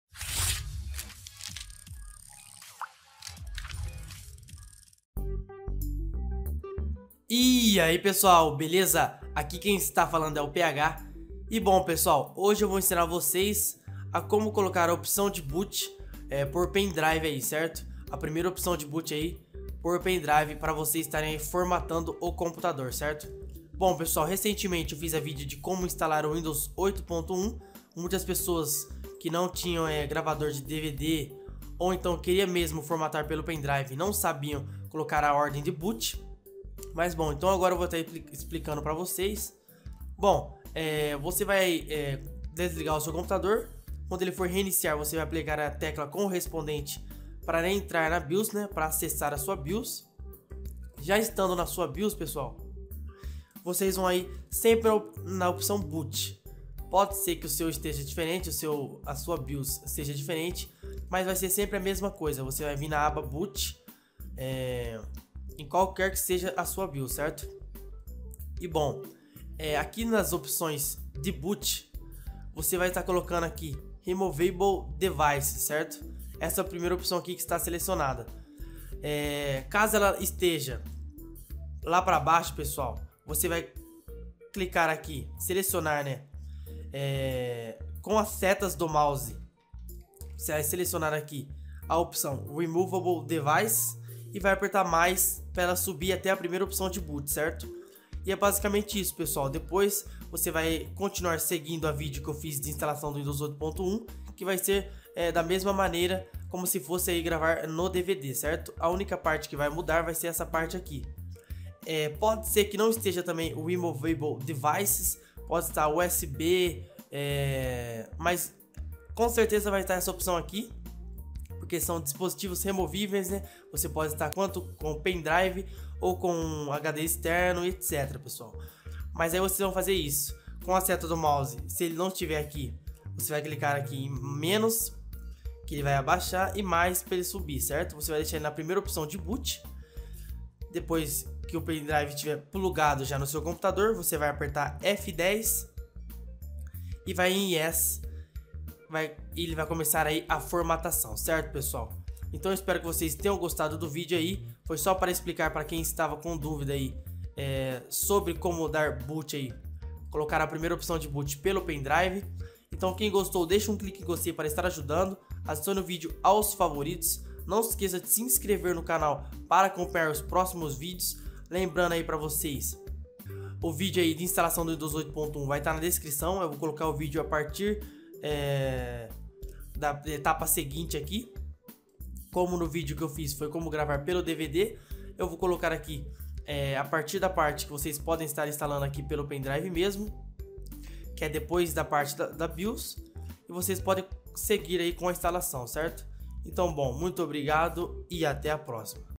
Como configurar ordem de BOOT para pendrive (USB)
Ensino como configurar a BIOS para dar BOOT por pendrive, uma coisa muito simples, o único problema é que com certeza sua BIOS não será igual, mas como disse o método será o mesmo.

Canal de Personalização do Windows